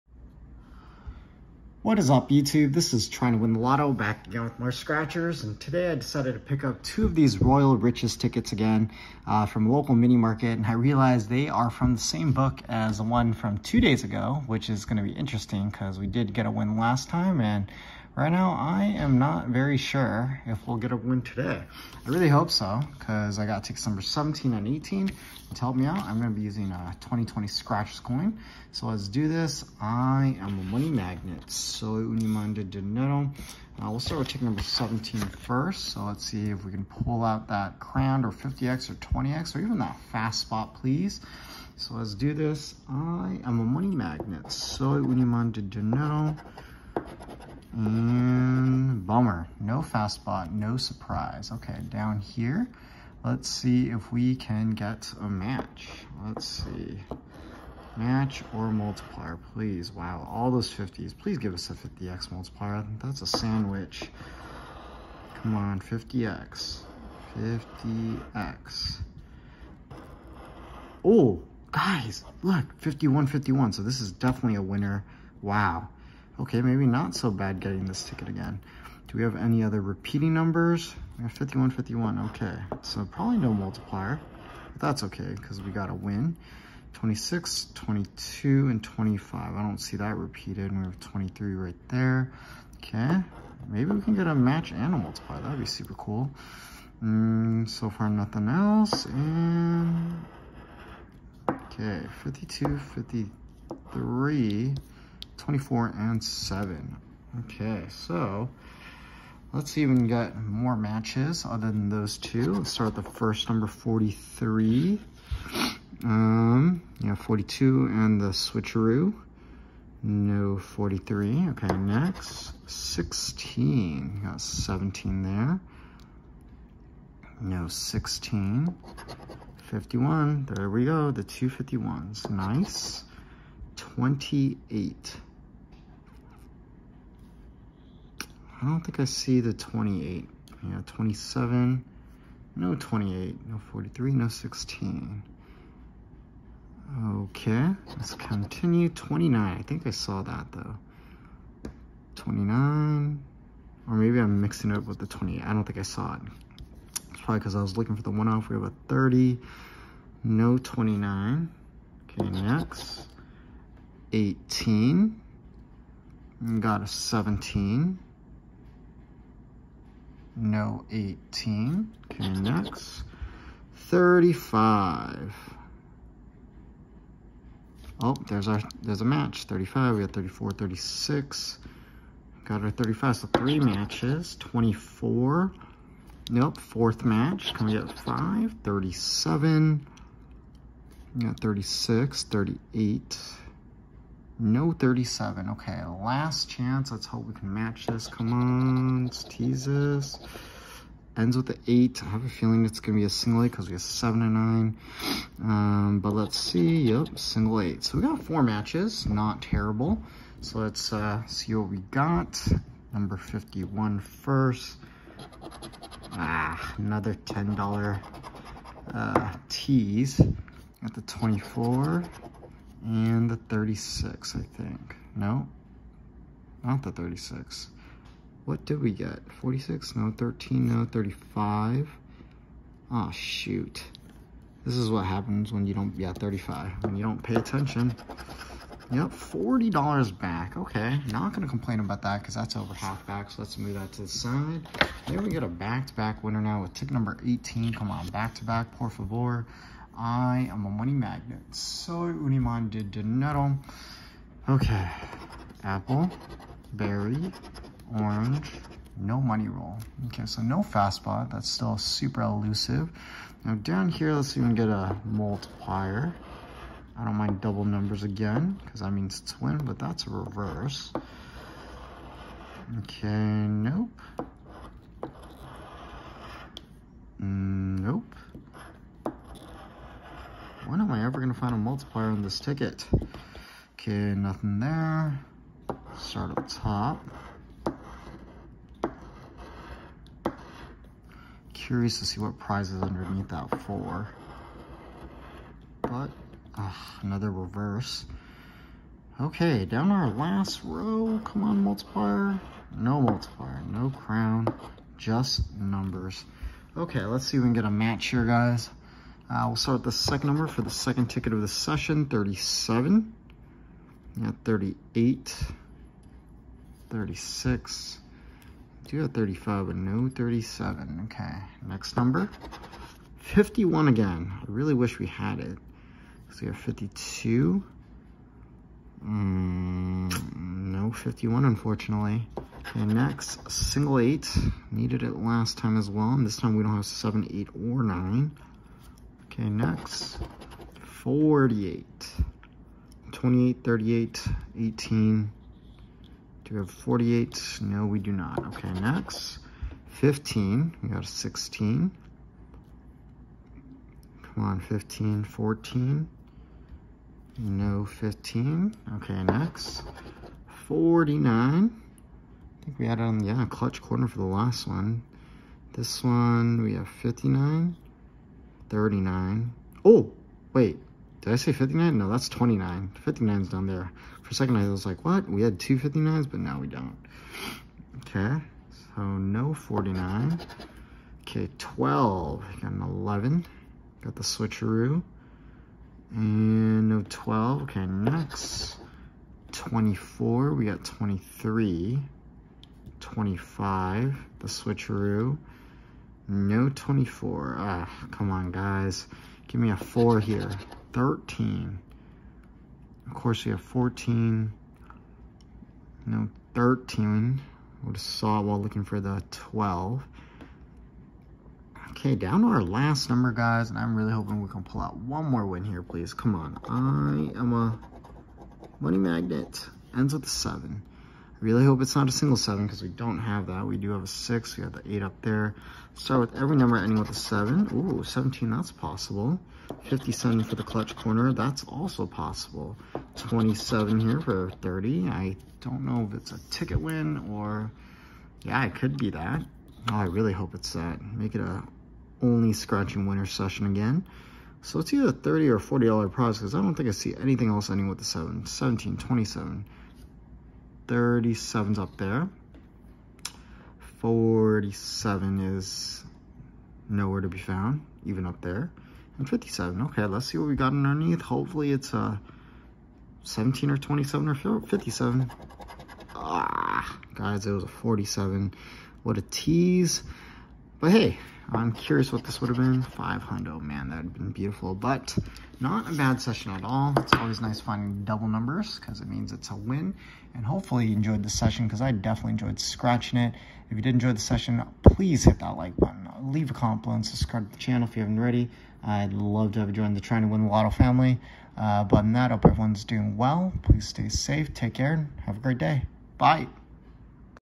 ANOTHER WIN ON ROYAL RICHES! 👑 CA Scratchers
Today is Day 4 of 7 of this series with $60 CA Scratchers! My friends and I are investing more money to get more tickets with the hope of hitting a jackpot!